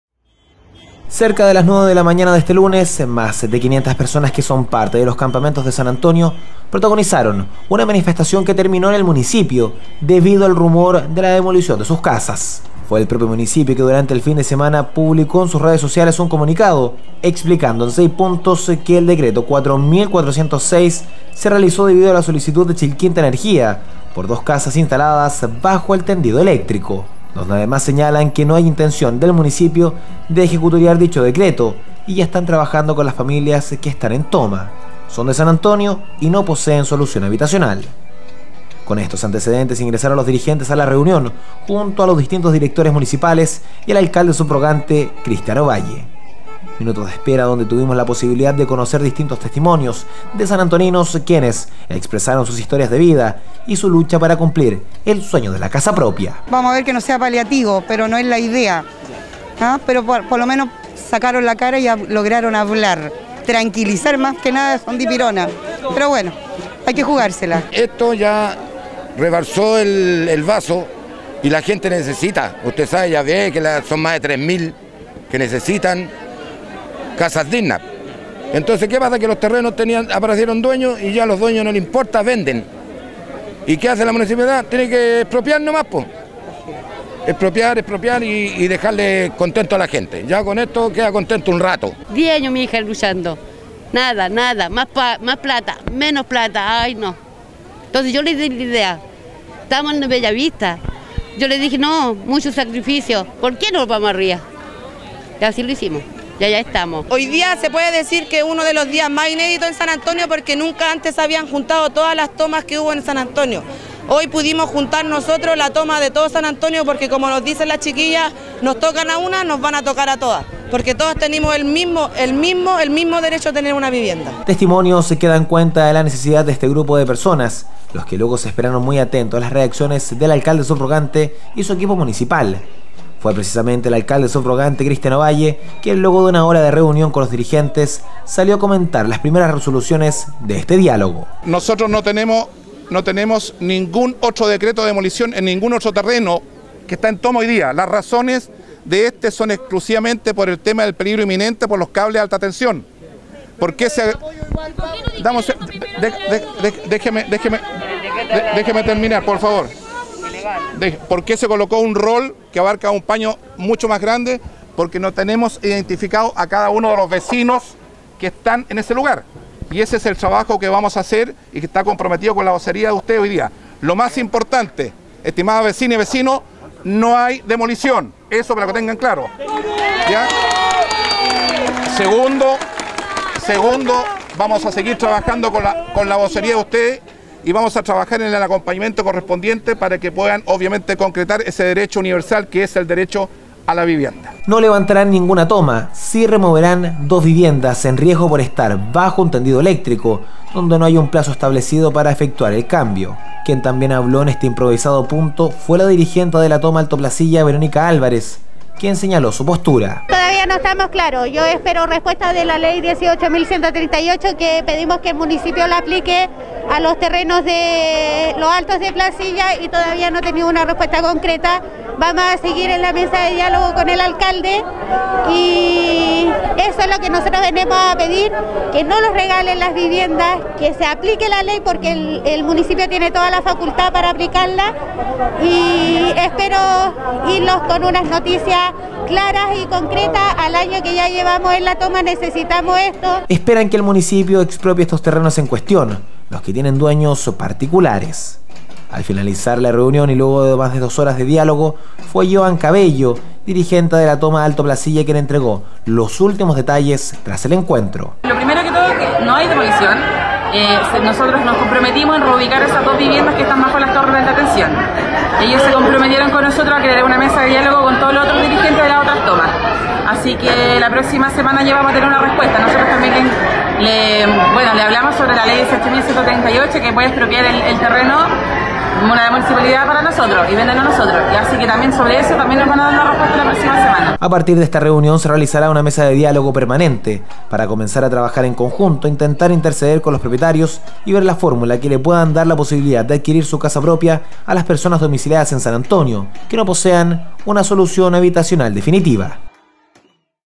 23 NOV 2020 HABITANTES DE LAS TOMAS PROTESTARON EN EL MUNICIPIO
MÁS DE 500 PERSONAS QUE SON PARTE  DE LOS CAMPAMENTOS DE SAN ANTONIO PROTAGONIZARON UNA MANIFESTACIÓN QUE TERMINÓ EN EL MUNICIPIO, ESTO DEBIDO AL RUMOR DE UN EVENTUAL DESALOJO POR UNA PUBLICACIÓN EN LAS REDES SOCIALES. EL ENCUENTRO TERMINÓ CON LA CONFIRMACIÓN DEL ALCALDE SUBROGANTE QUE NO HABRÁN DESALOJOS Y DESDE AHORA HABRÁ UNA MESA DE DIÁLOGO, YA QUE LOS DIRIGENTES DE LAS TOMAS ESPERAN QUE EL MUNICIPIO EXPROPIE LOS TERRENOS BAJO EL ALERO DE LA LEY 18.138.